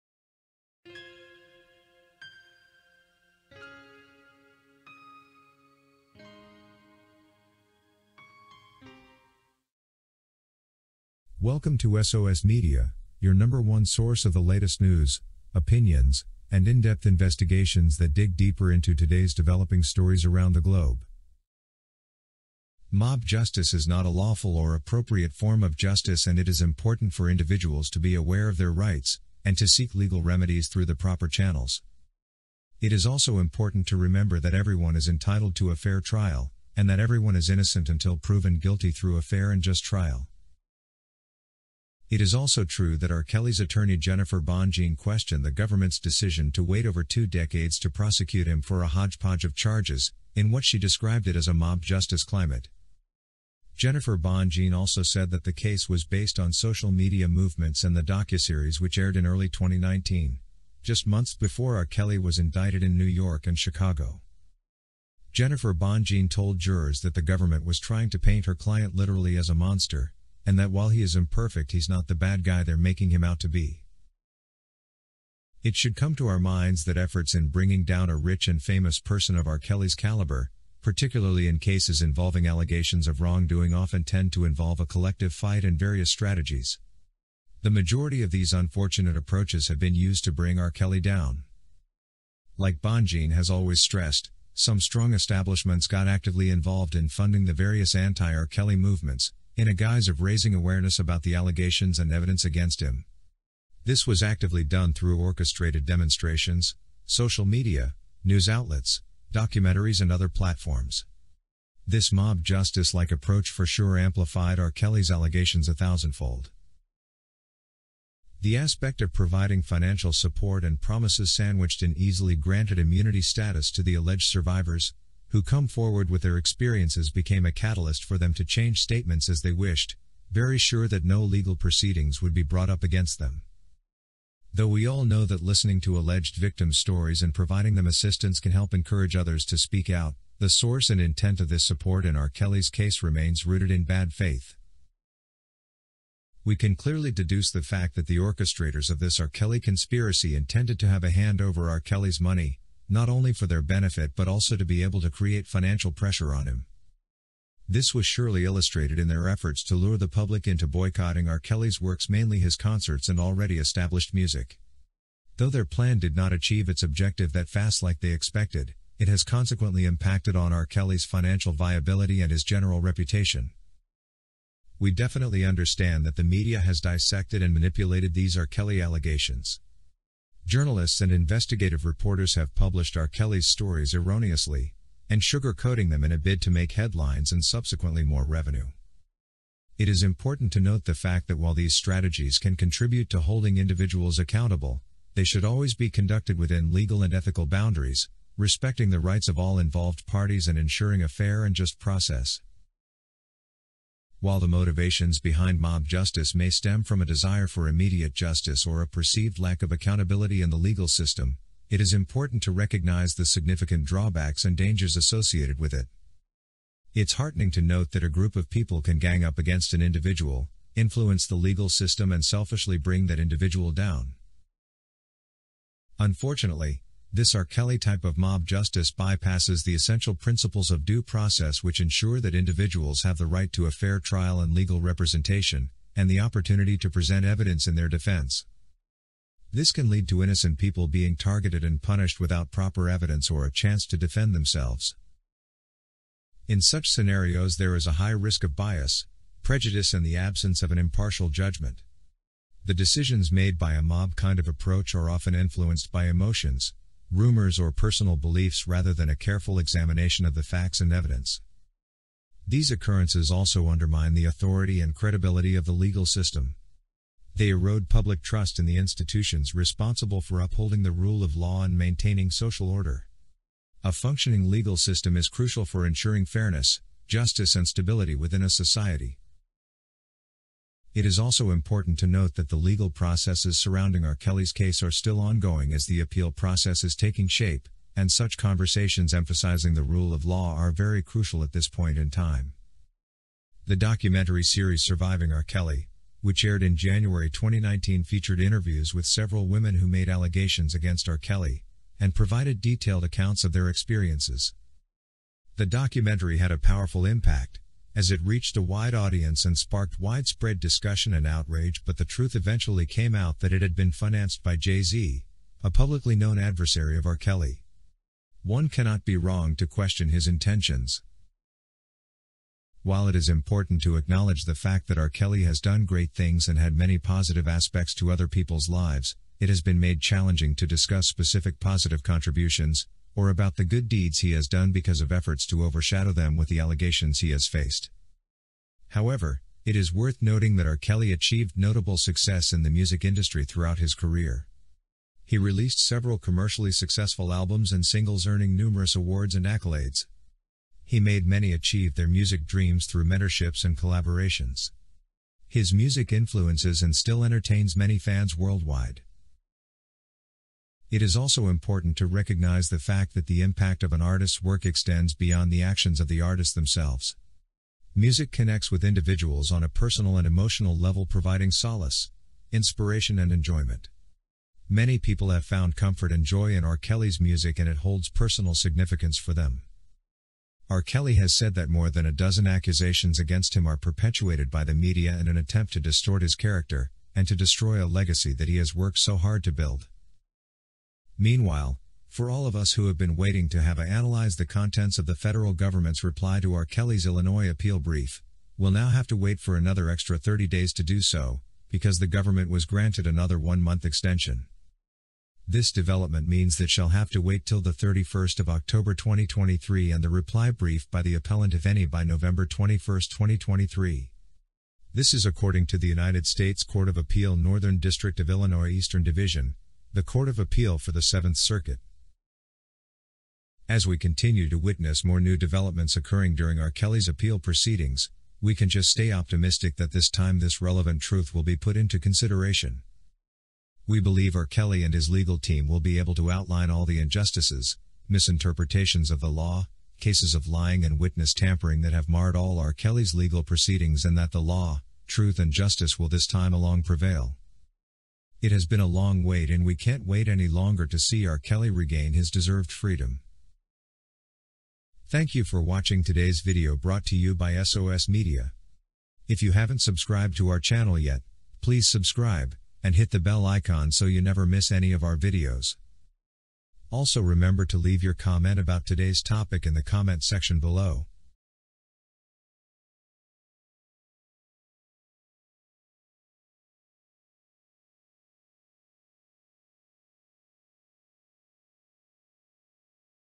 R Kelly will prevail over mob justice - Bonjean
It is also true that R Kelly's attorney Jennifer Bonjean questioned the government’s decision to wait “over two decades” to prosecute him for "a hodgepodge of charges" in what she described it as “a mob justice climate.” Jennifer Bonjean also said that the case was based on social media movements and the docuseries, which aired in early 2019 — just months before R Kelly was indicted in New York and Chicago.